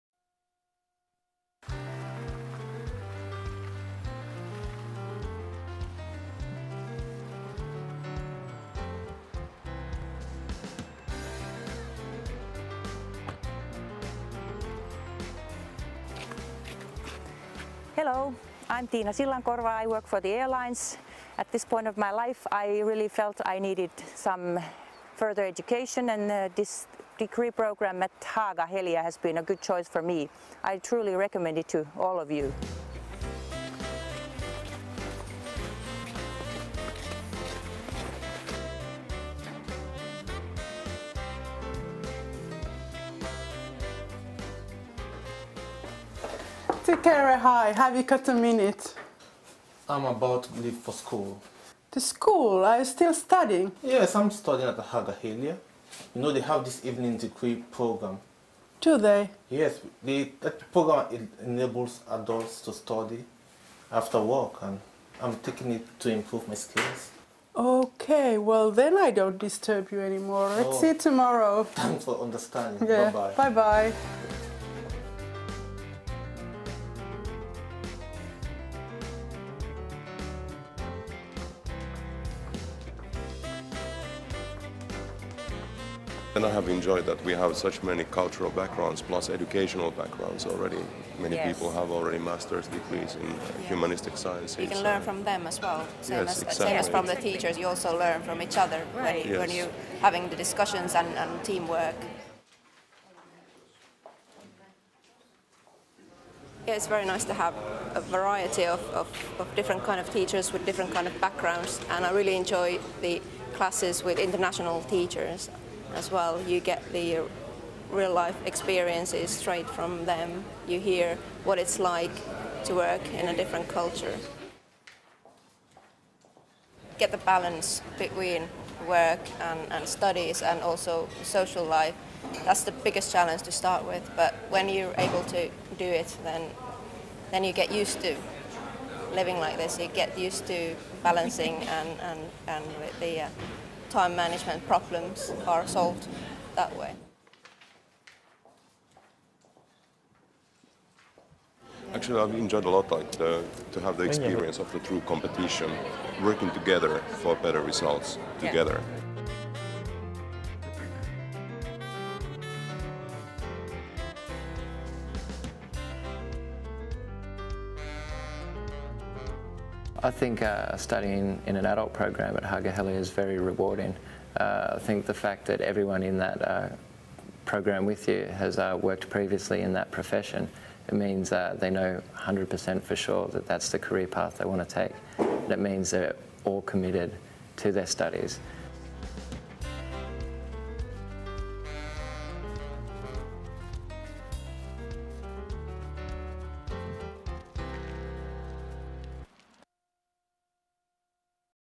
HAAGA-HELIA Adult Education
This video gives you a snapshot introduction to the adult studies in English at HAAGA-HELIA University on Applied Sciences